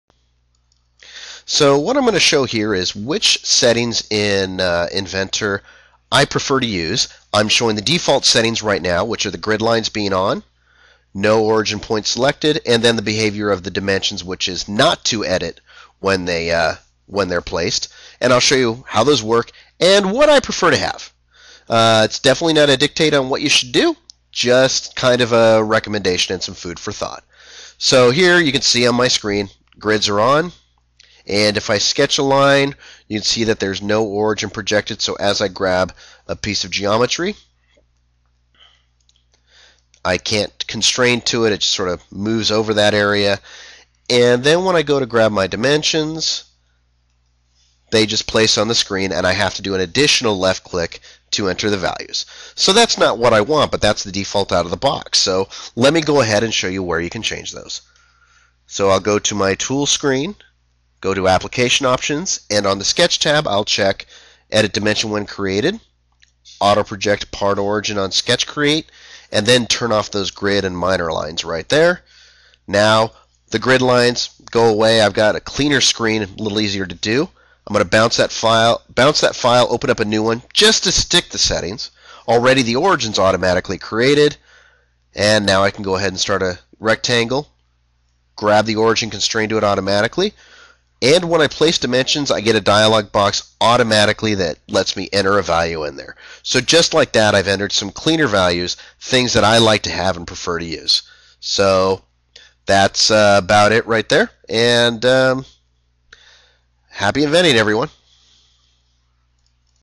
Ketiv Autodesk Quick Tip - Default Inventor Settings
A brief video describing the sketch settings that I change whenever I install a new seat of Inventor.  These are just my preferences.  Prefer to use whatever you want.